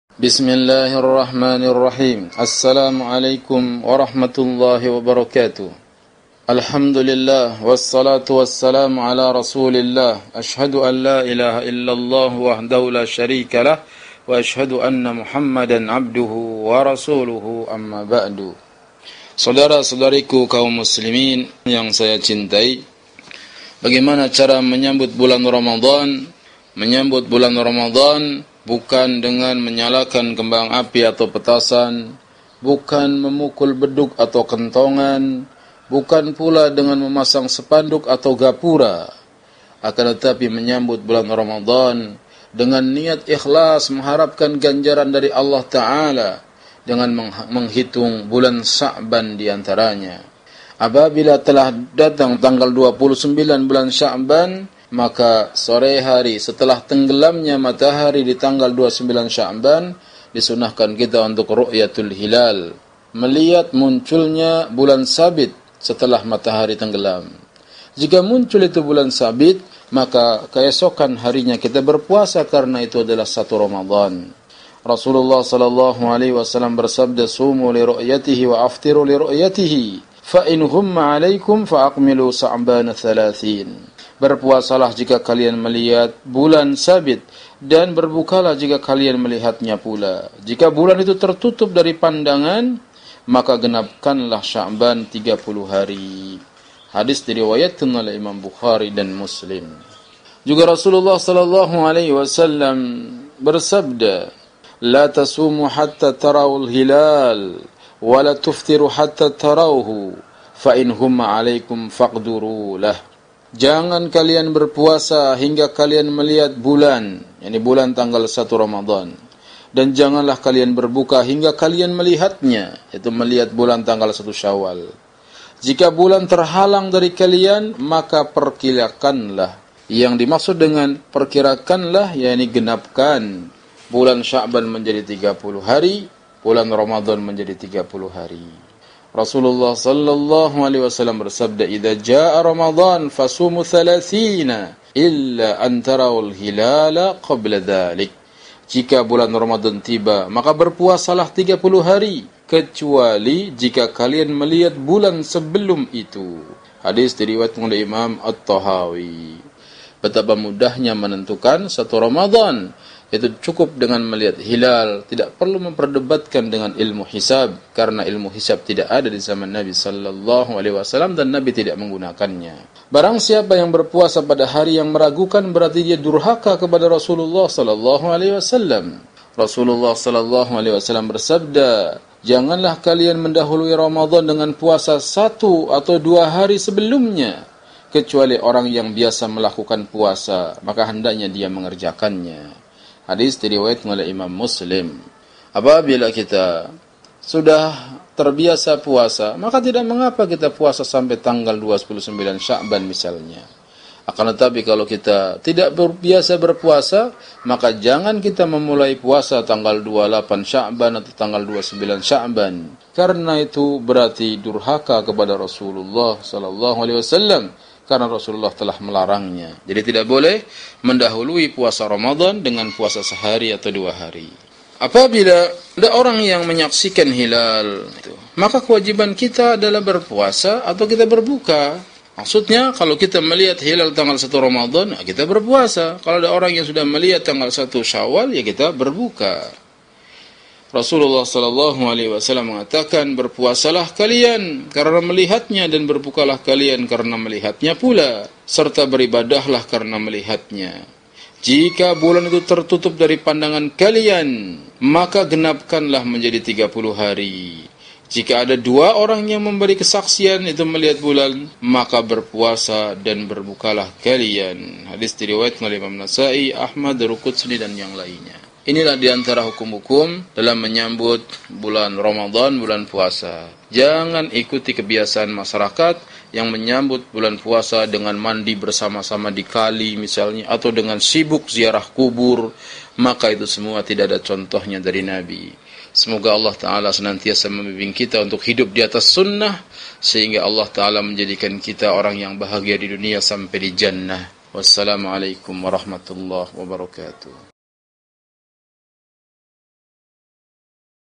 Sarapan Pagi eps 05 : Menyambut Ramadhan - Ust Sulam bin Mustareja
Episode 05
Tema : Menyambut Ramadhan
Tausiyah Singkat - Sarapan Pagi bersama Ustadz Sulam Mustareja, Grup WA Assunnah Gateway.